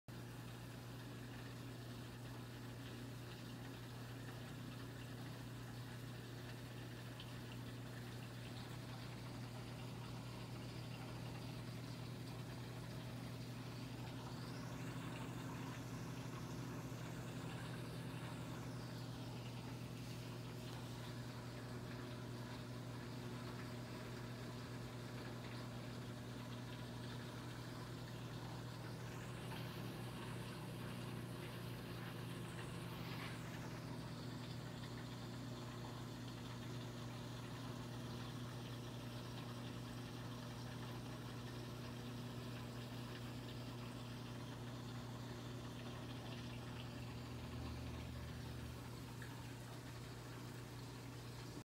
Australian spider sponge
7 month old from LFS fishy business is TucsonAZ. Beautiful Australian spider sponge.